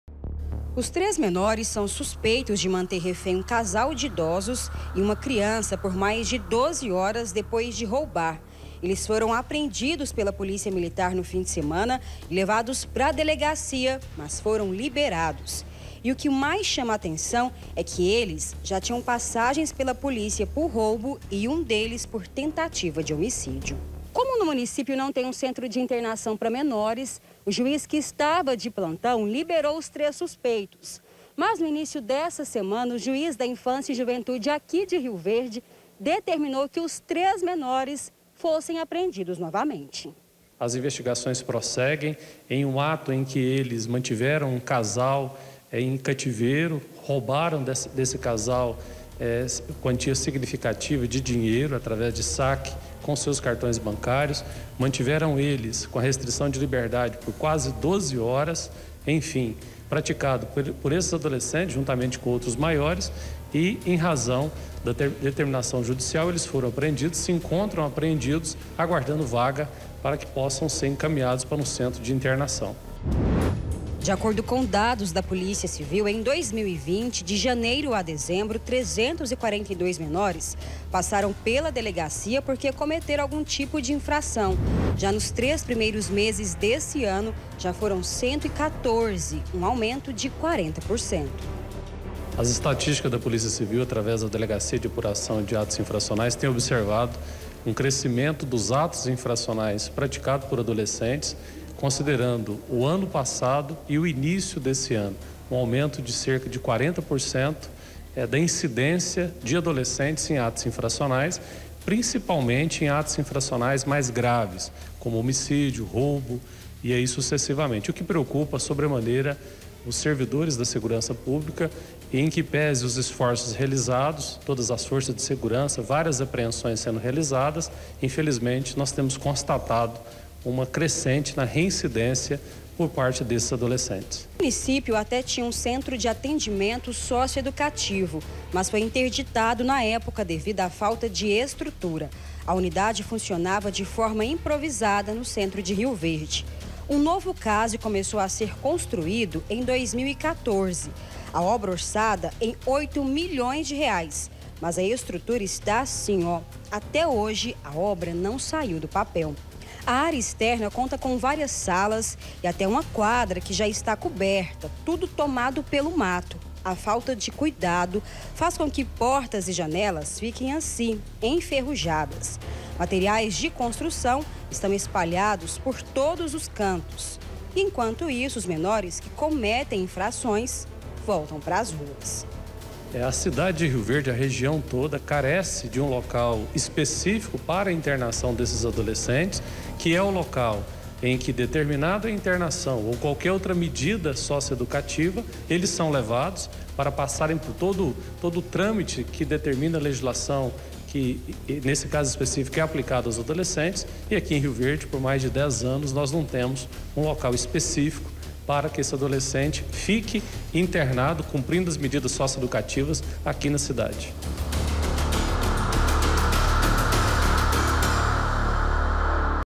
LIBERA GERAL: FALTA LUGARES PARA MENORES INFRATORES
E OLHA SÓ, O QUE A FALTA DE CENTRO DE INTERNAÇÃO PARA MENORES INFRATORES ESTÁ FAZENDO NO ESTADO. NA CIDADE DE RIO VERDE, OS ADOLESCENTES QUE COMETERAM INFRAÇÕES ESTÃO SENDO LIBERADOS POR NÃO TER ONDE FICAR. NO MUNICÍPIO SÓ NOS TRÊS PRIMEIROS MESES DESTE ANO, 114 JOVENS FORAM APREENDIDOS NA CIDADE.